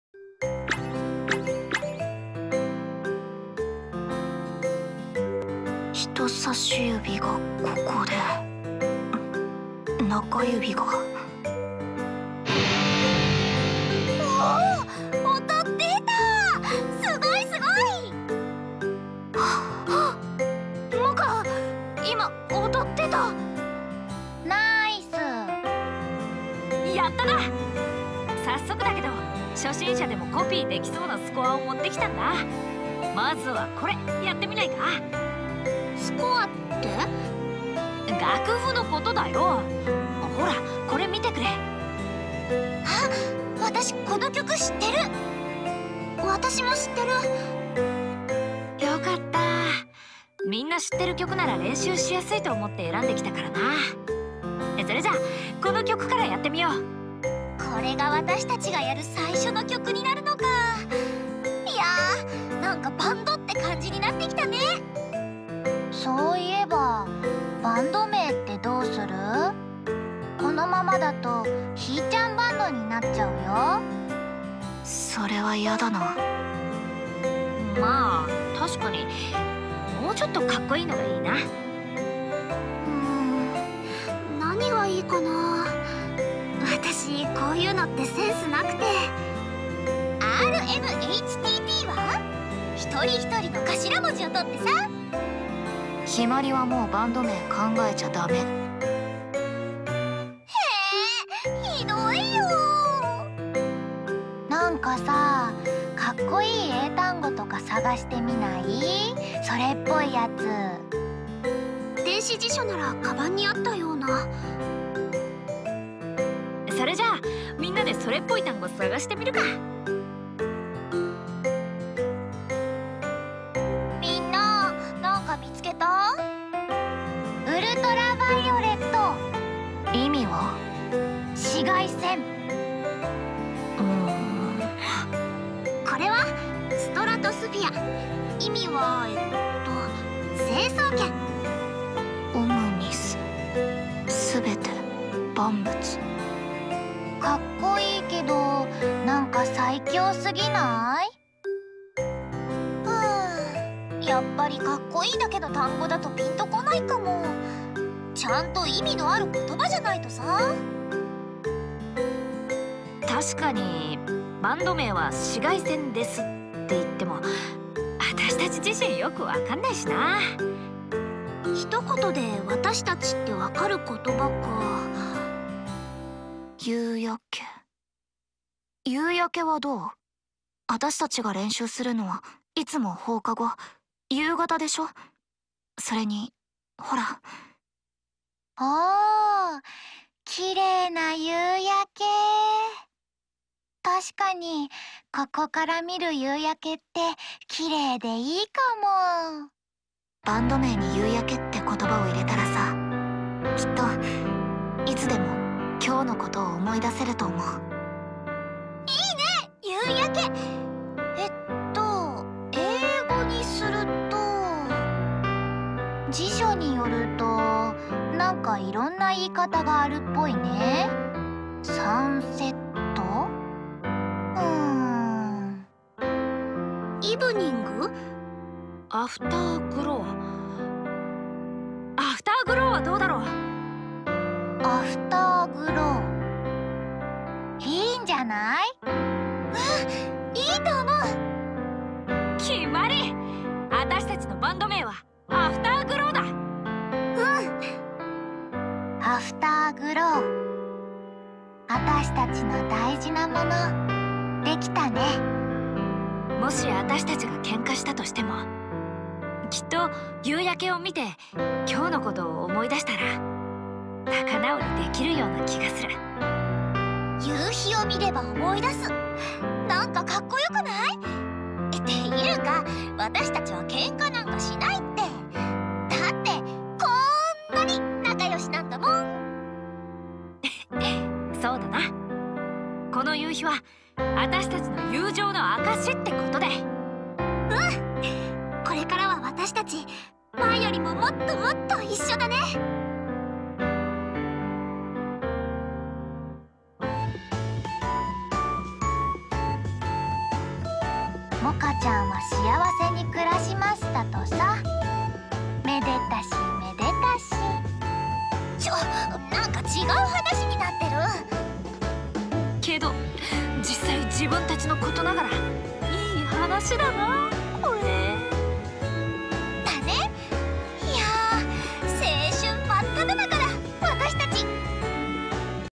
As Evening Clears Chapter 5: Where We All Belong
Now that they have a band, the girls need to come up with a name. The search for cool words begins.